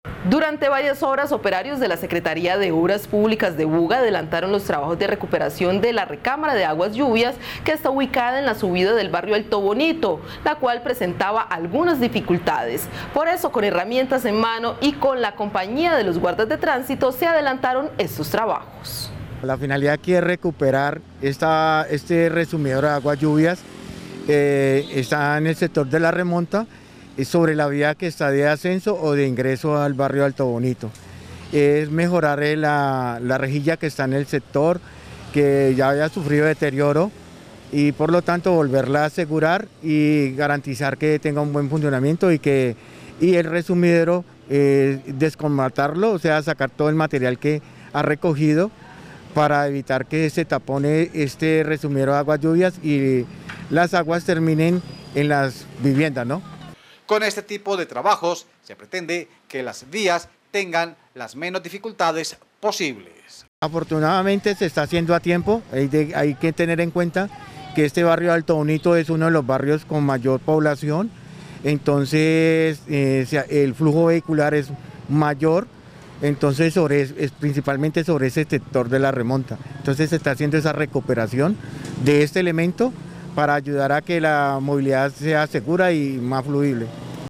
Recuperación de recamara, Alto Bonito
Durante varias horas, operarios de la secretaria de obras públicas adelantaron los trabajos de recuperación del quiebrapatas, ubicado en la subida del barrio Alto Bonito de Buga, el cual presentaba algunas dificultades. Por eso, con herramientas en mano y con la compañía de los guardas de tránsito se adelantó este trabajo.